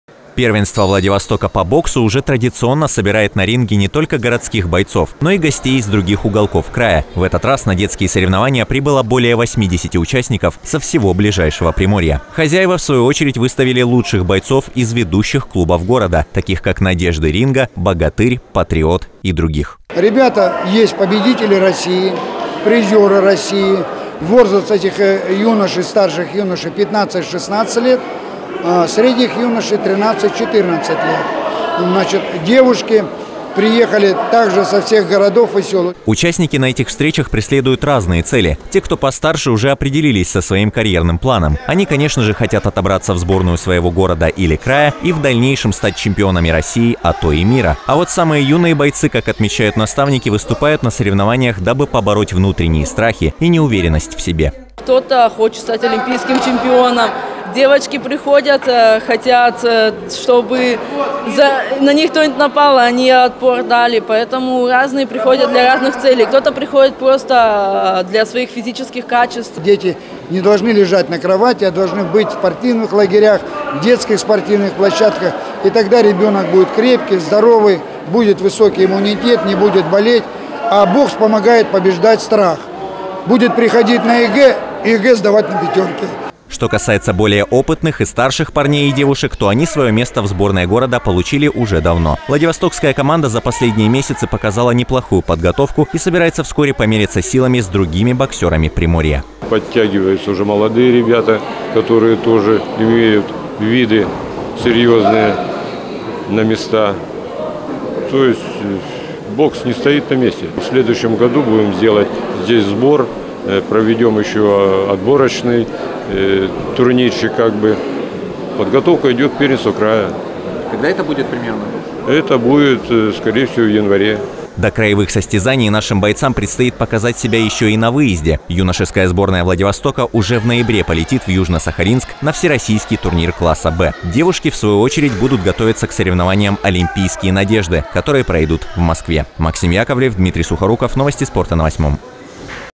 Первенство города по боксу среди юношей и девушек прошло во Владивостоке
Юные боксеры Владивостока встретились на первенстве города. Детские соревнования собрали более 80 бойцов из различных клубов приморской столицы. На ринге встречались как мальчики, так и девочки.